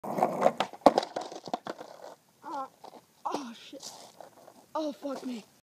Skate fail: Busting Balls
I was skating a concrete slab near my house and this happened...